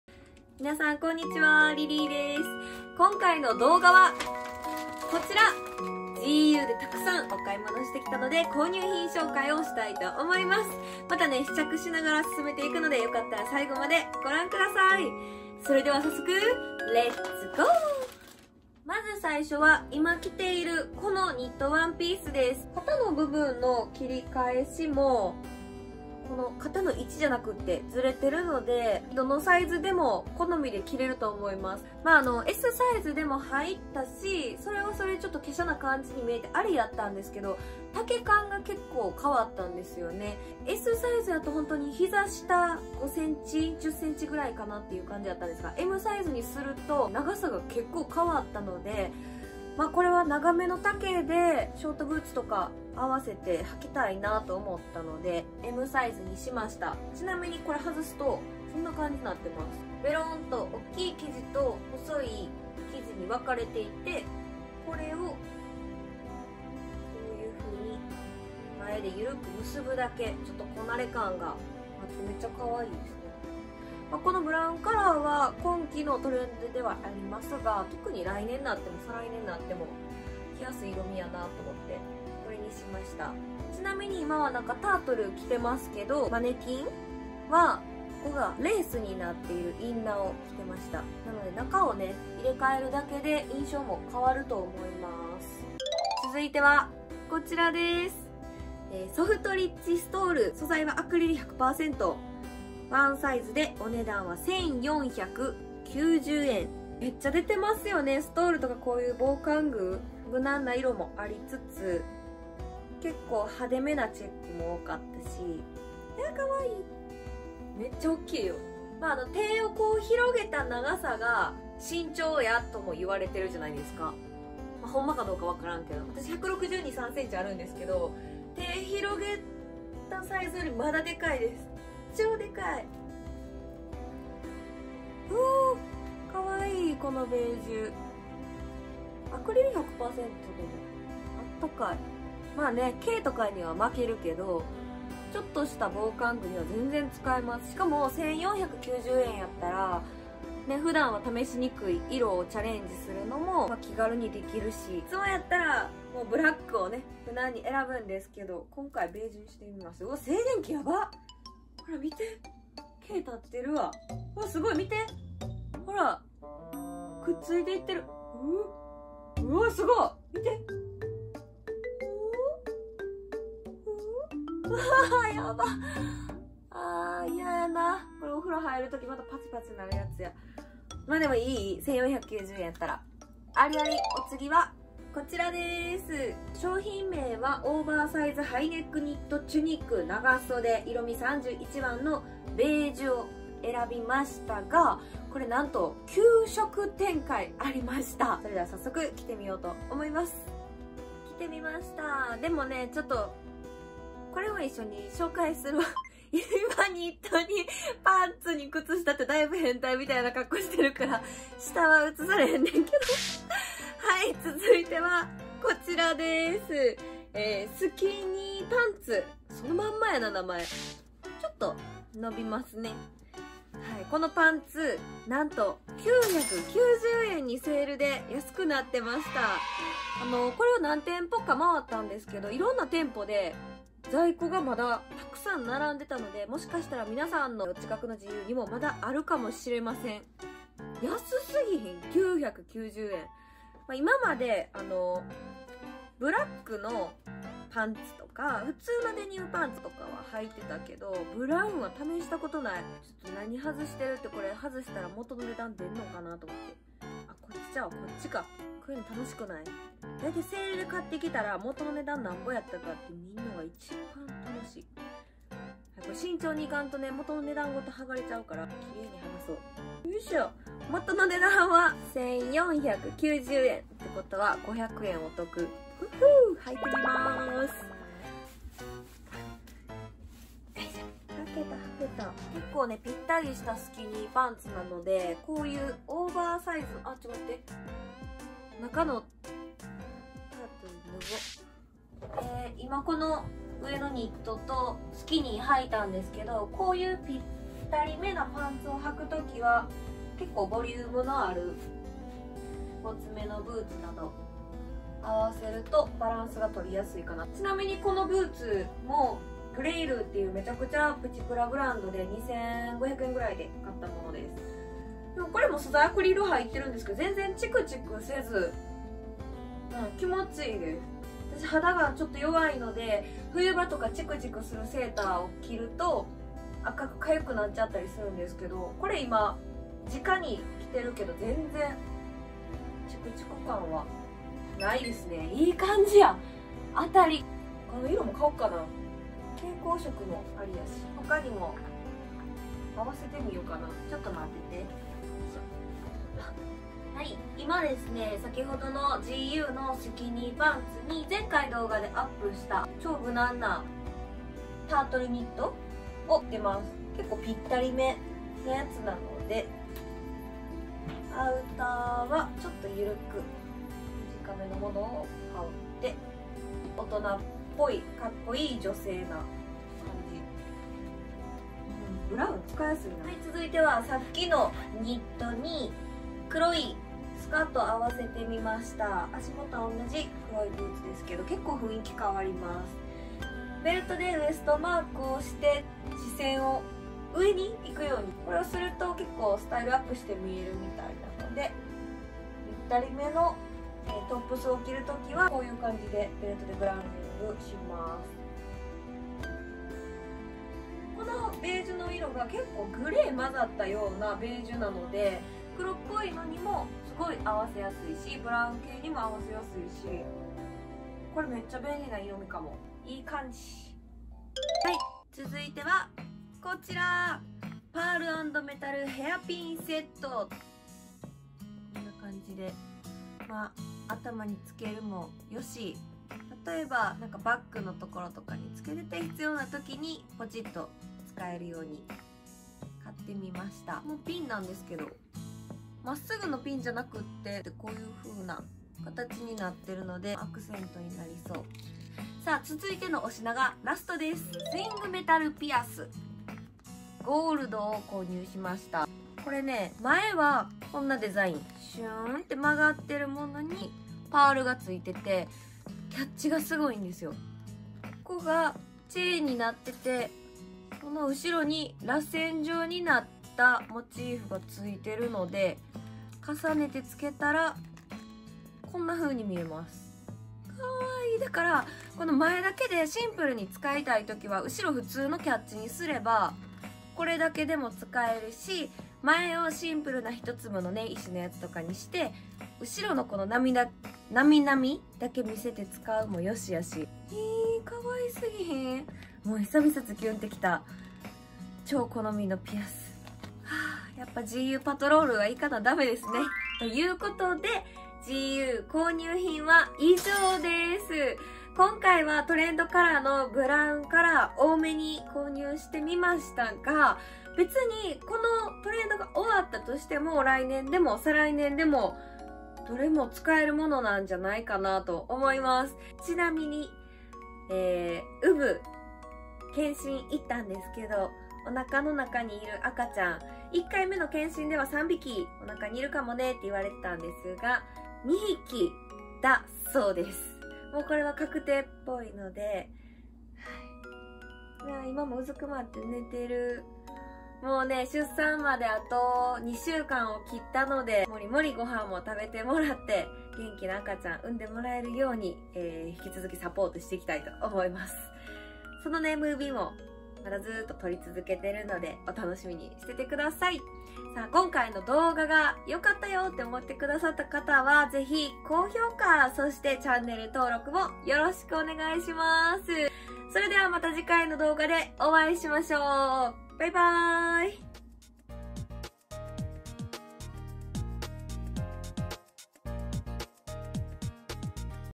【GU購入品】秋冬の大人可愛い着回しコーデ♡
今期の新作ほんま可愛すぎてやばい！アクセも洋服もお気に入りがまた増えたわ♡いや〜やっぱ良いお買い物した後の気分って最高！